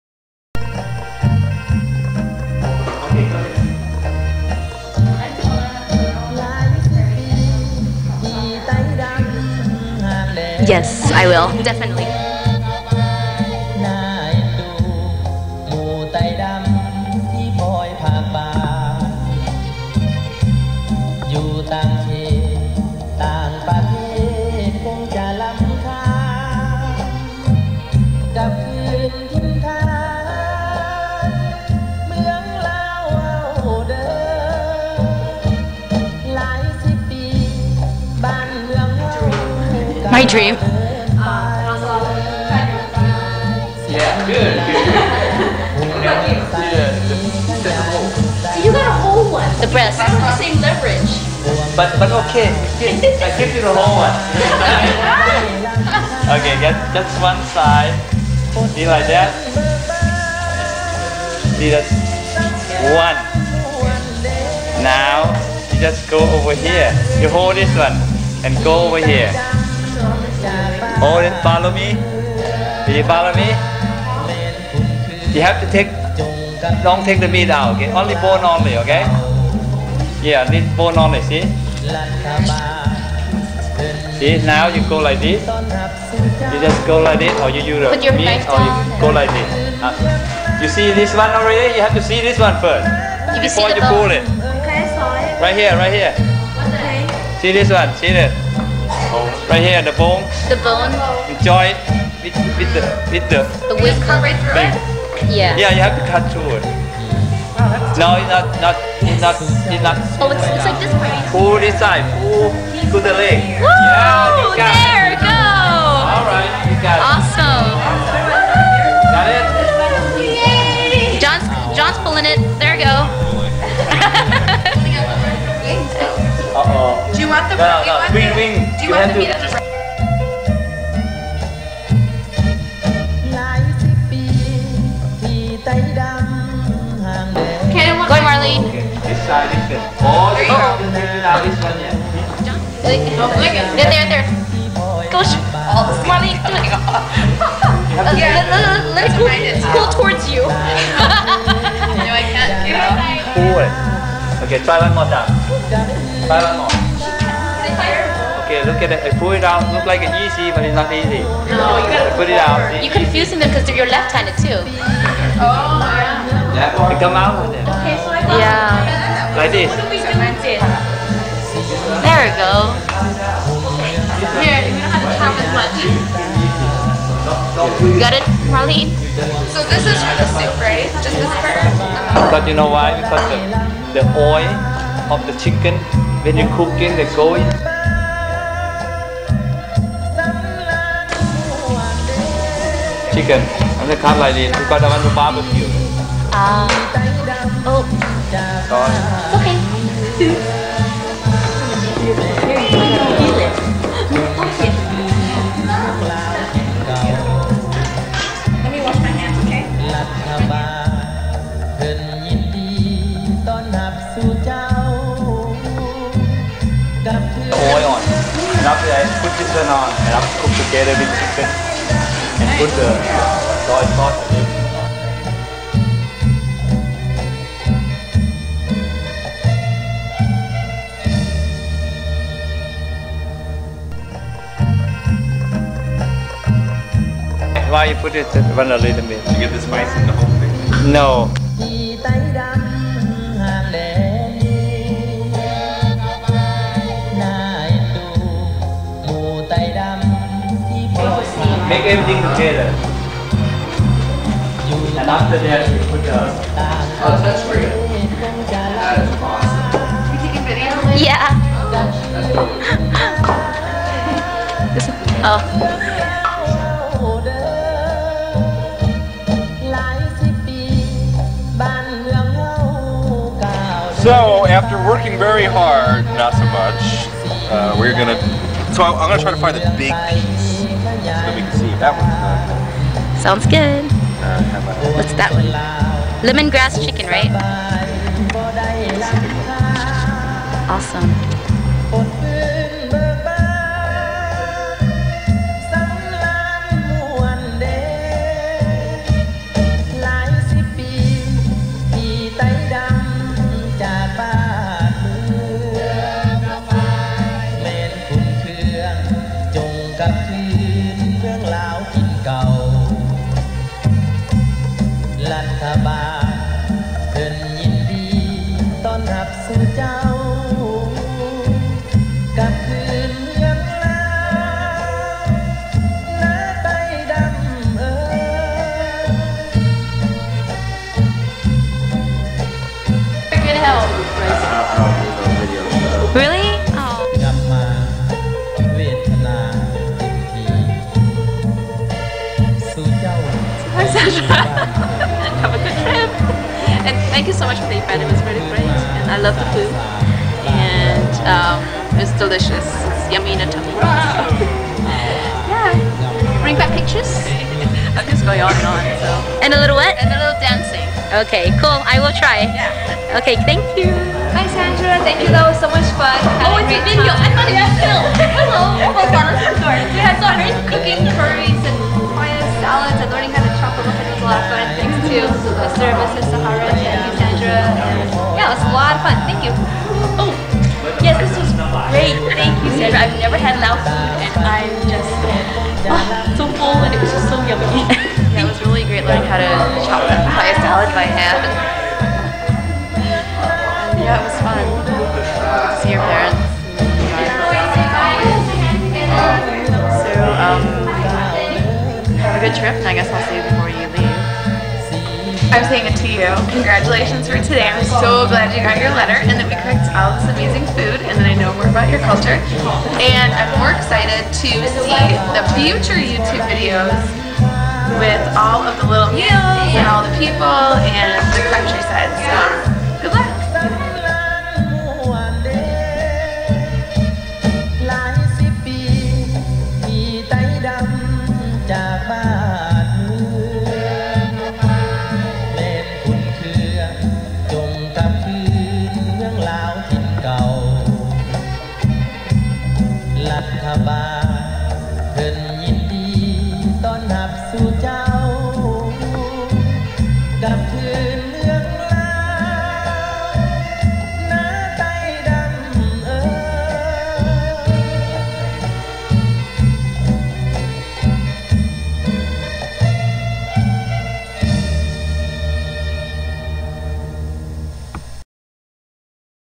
Exploring Laos with Us "Lao Cooking Session" Part 5
My first Lao Cooking Session.  It was lots of fun.  This is one of the ways I fundraised for my Study Abroad trip to Laos.  I had to help out my parents...so I didn't really have a chance to film more...sorry.  Follow me to help fund my way to Laos!